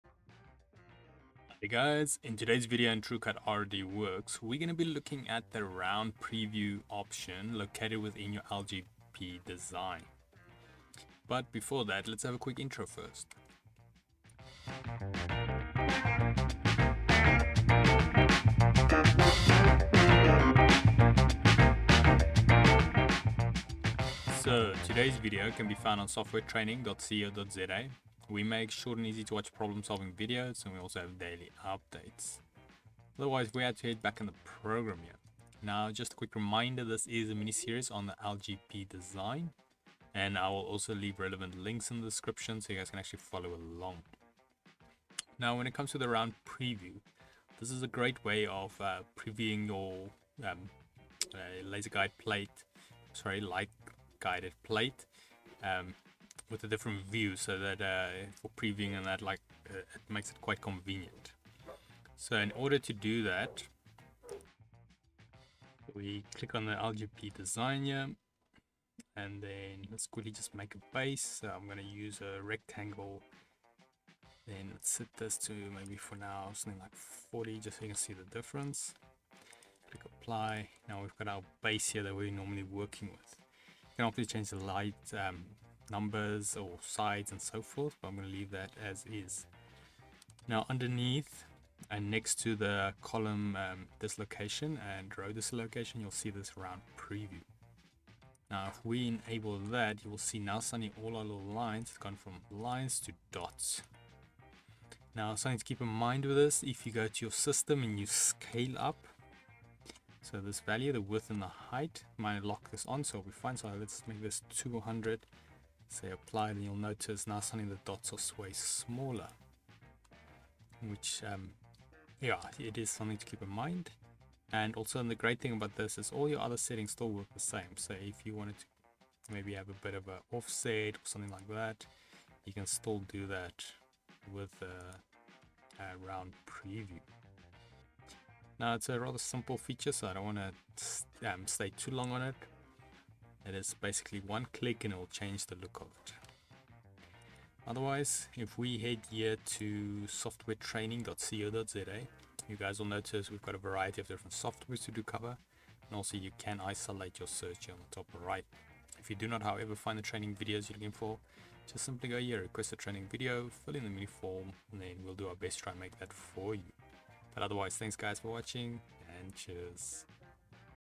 TruCUT RDWorks the LGP Round Preview Option (part 11) LGP Design Videos Mini-Series
The Round Preview option is a nice fresh look when you are working on the LGP Design Feature.

It is also good to note that when you are working with the Round Preview enabled, that all the other settings will still work very similar.

This video is part of a Mini-Series. Next video covers LGP Design – Circle Type. Previous video covered LGP Design – Row and Col Dislocation.